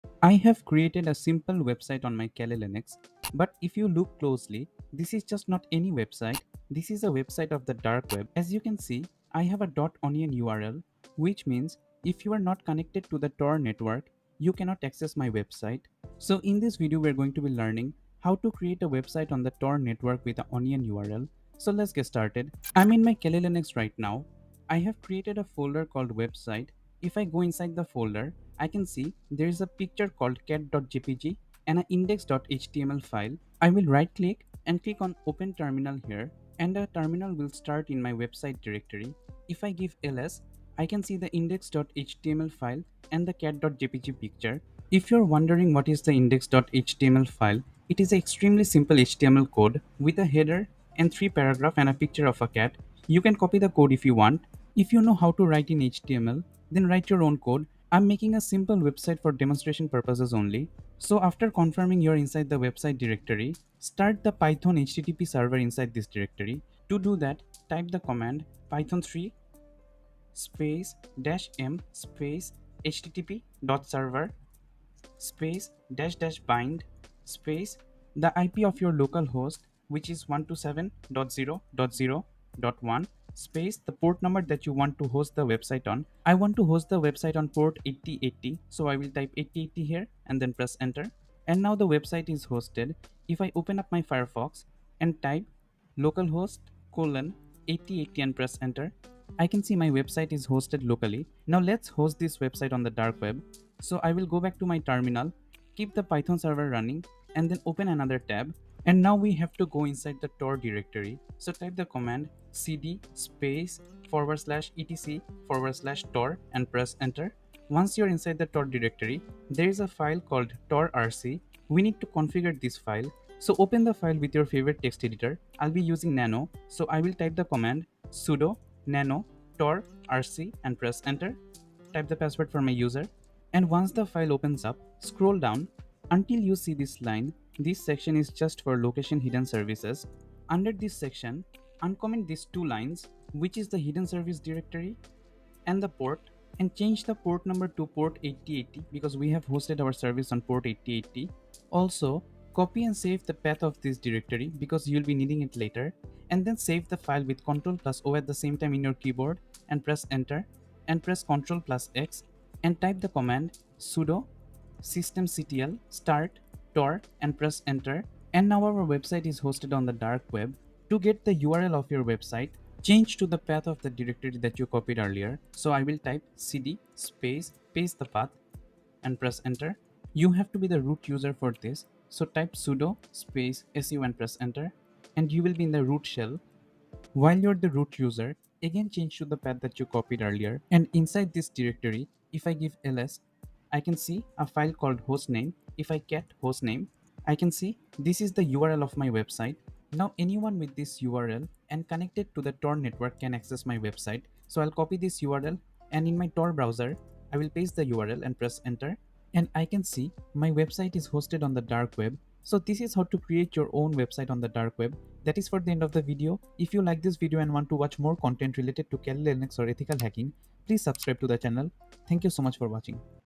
Create your own Website on DARK WEB! (with .ONION URL)
In this video, we will learn how to create a website on the Dark web (Tor network) with a .onion address.